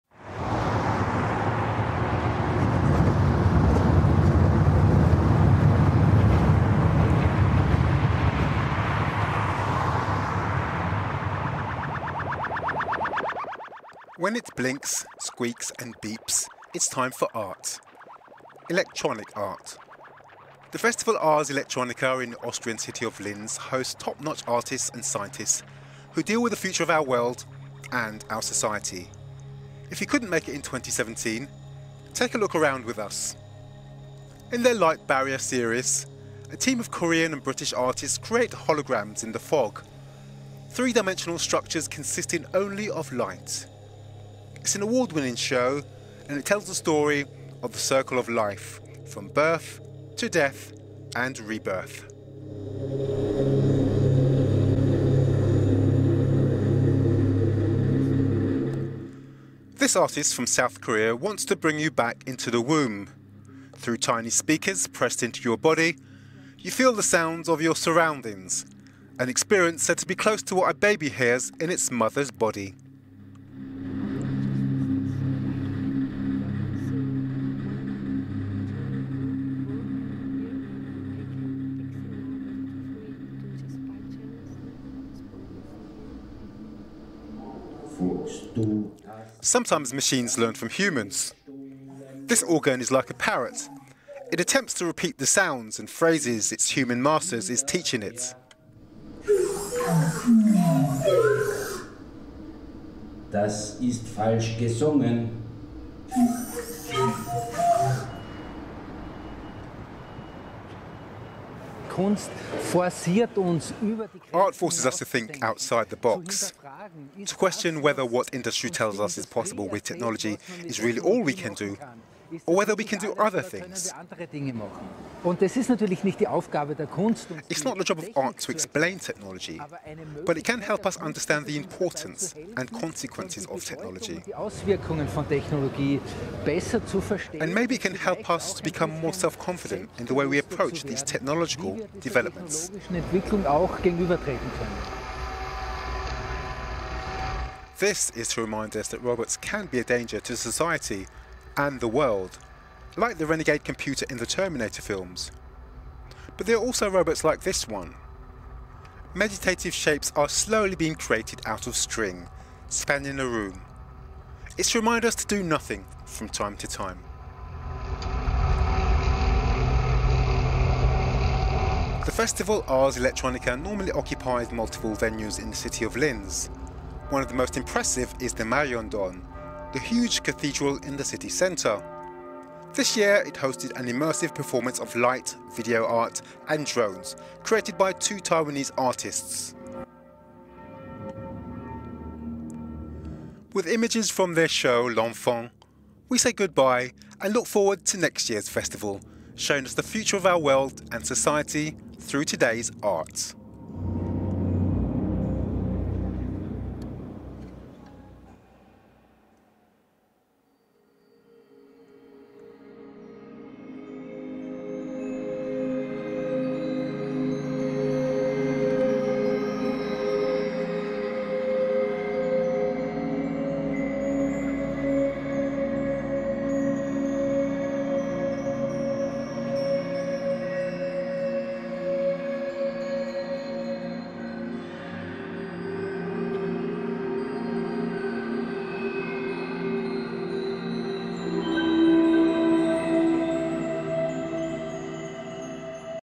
Thinking outside the box at the Festival Ars Electronica
When it blinks, squeaks and beeps, it is time for Art. Electronic art. The Festival Ars Electronica in the Austrian city of Linz hosts top-notch artists and scientists, who deal with the future of our world and our society.
If you could not make it in 2017, take a look around with us.